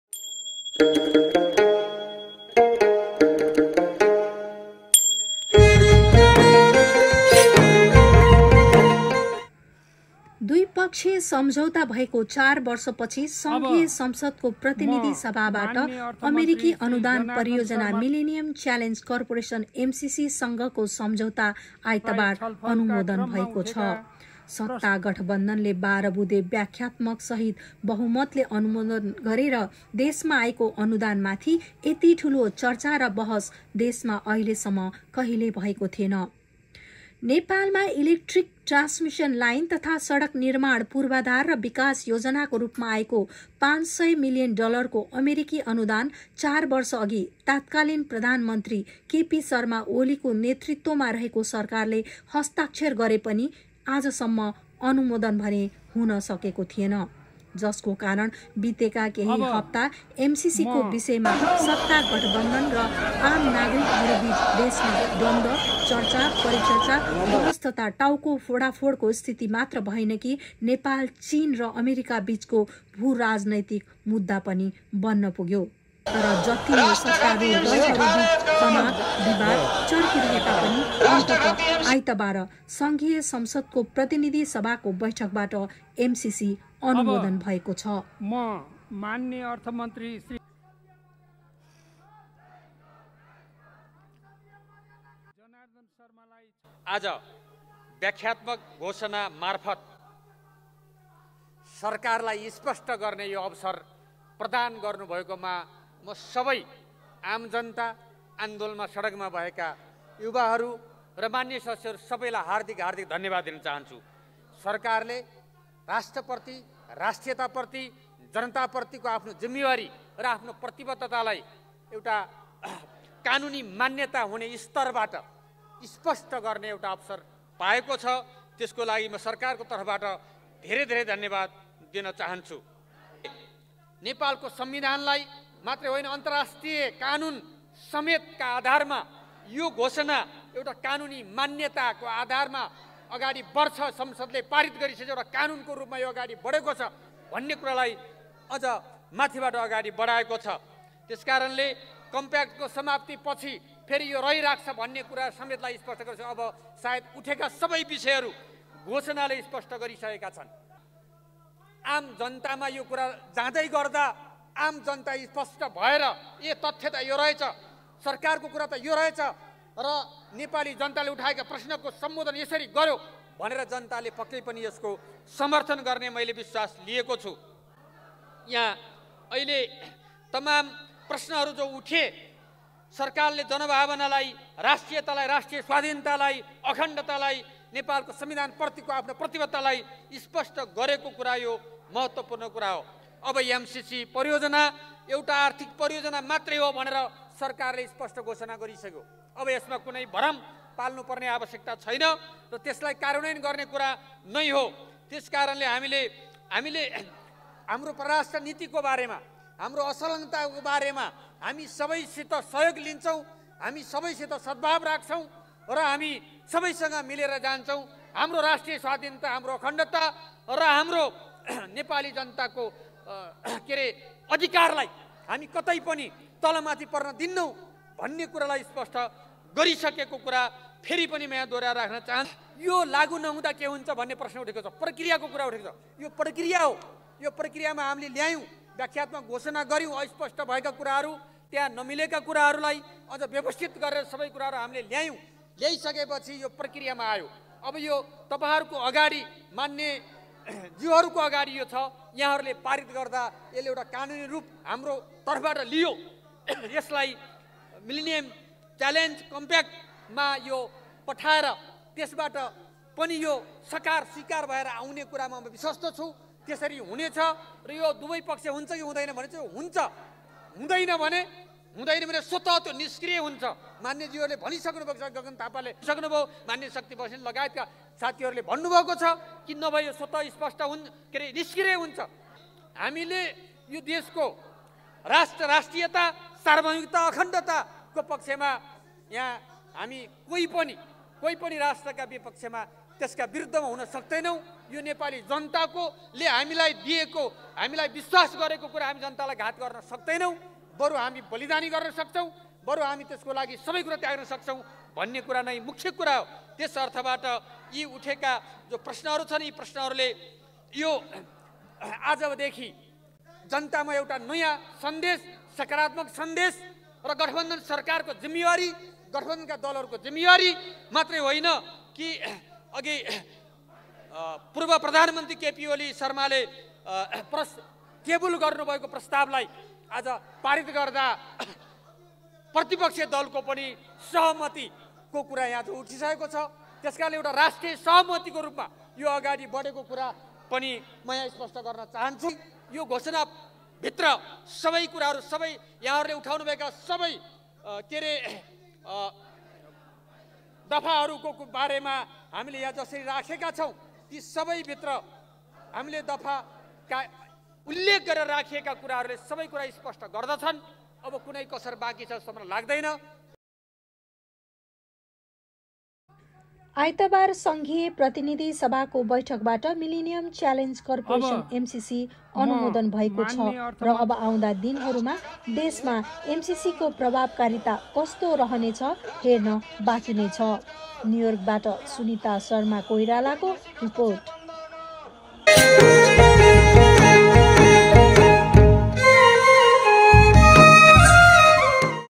Nepal Parliament Ratifies MCC Compact By Sunita Sharma koirala
नेपालमा संघिय प्रतिनिधि सभाको बैठकबाट MCC अनुमोदन I